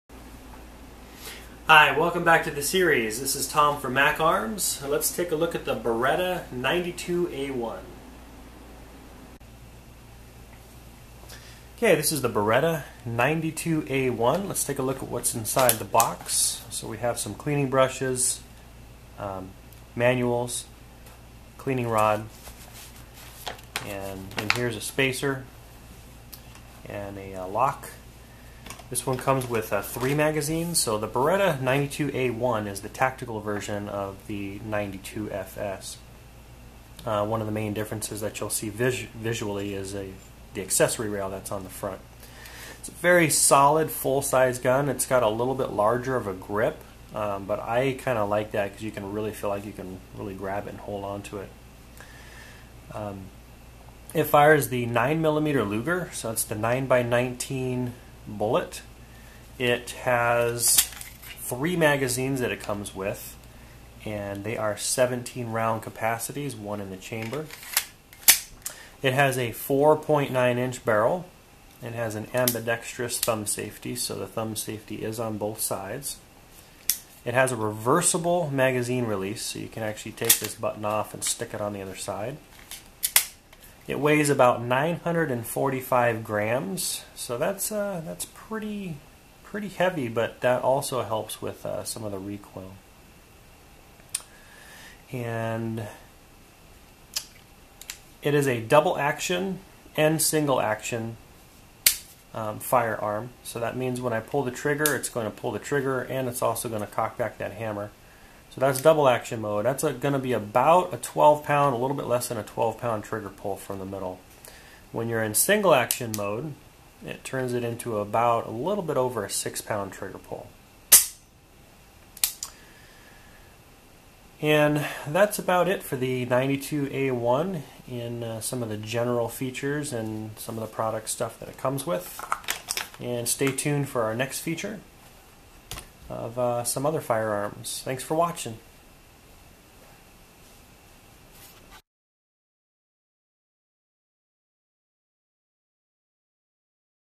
Inside the Box - Beretta 92A1 Handgun Features - Mac Arms LLC
Short Features of the Beretta 92A1 Handgun.  What's in the box, trigger pull, weight...